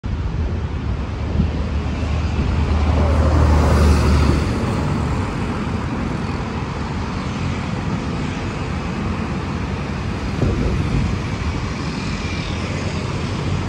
指南客運-248路線. 金龍汽車-KL6112U1. 【413-FW】
指南客運-248路線.
起迄站：[ 錦繡 ⇔ 民生社區 ]

拍攝地點：捷運松江南京站
拍攝時間：2023/02